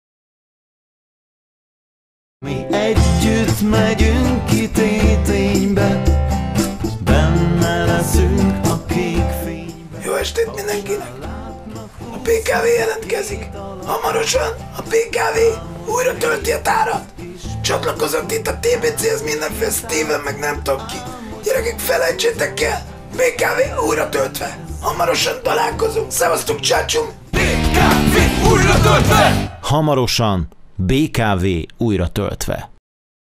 Előzetes II.
A BKV (BékásKovbojszVagyunk) legújabb dalának II. előzetese.